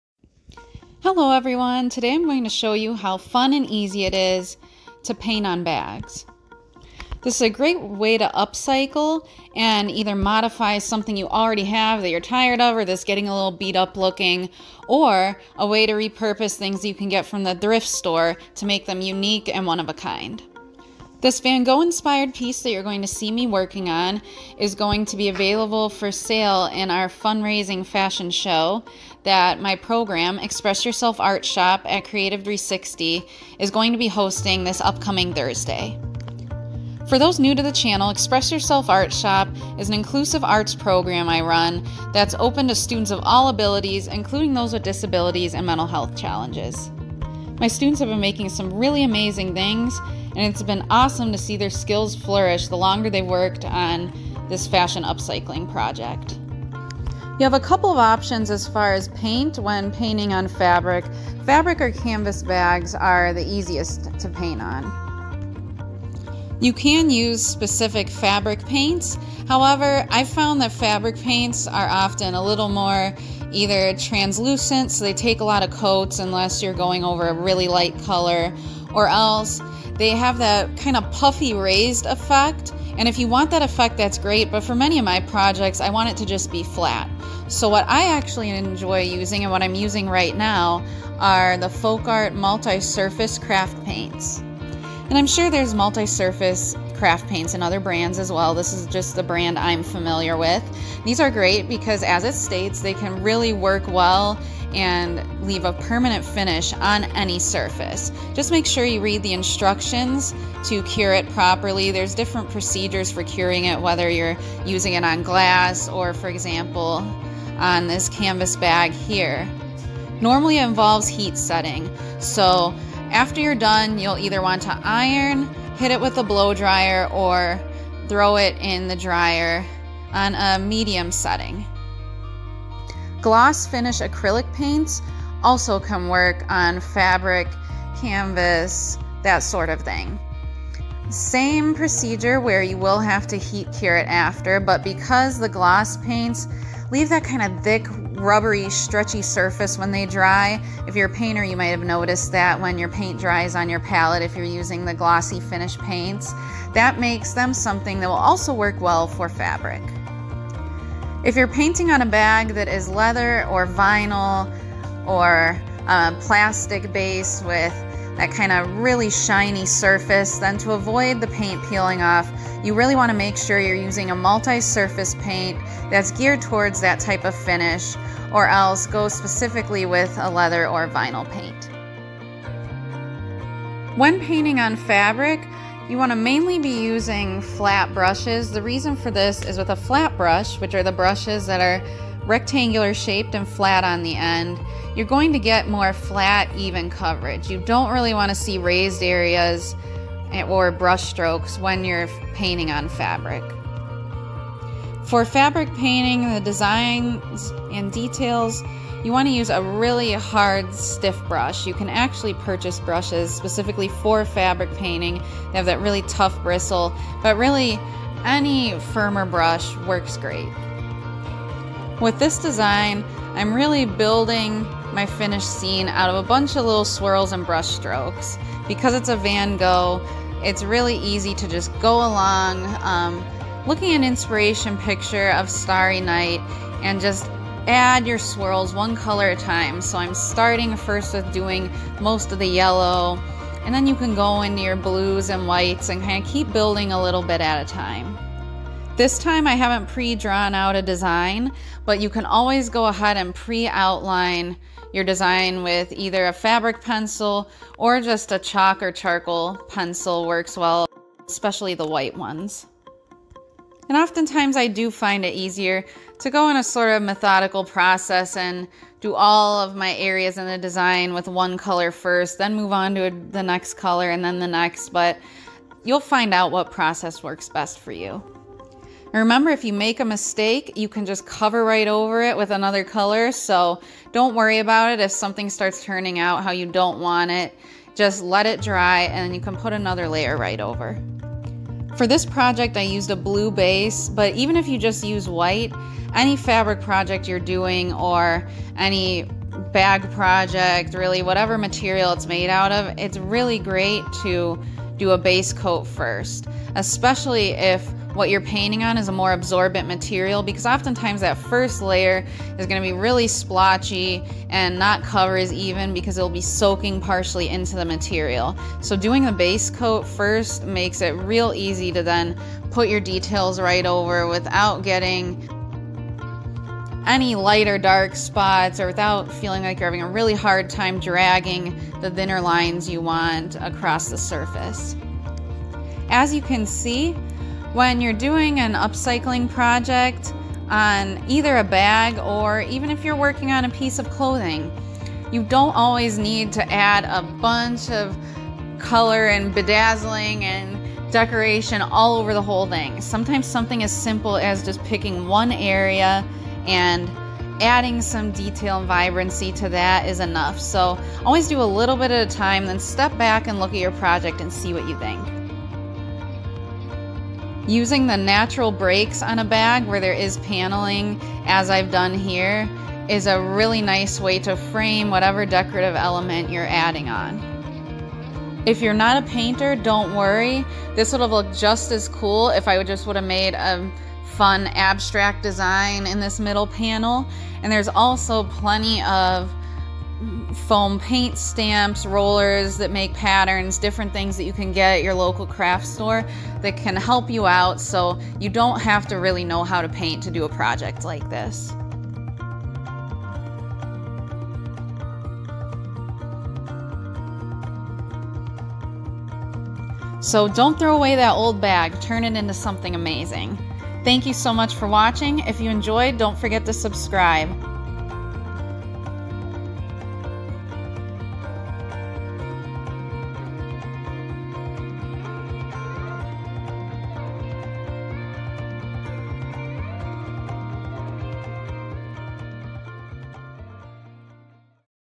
Painting On Handbags Tutorial - Starry Night Inspiration
Check out this demo as I decorate a messenger bag with a design inspired by Van Gogh's Starry Night.

Watch for tips on painting on handbags and clothing of various materials including what paints to use for different surfaces, the best types of brushes, and more!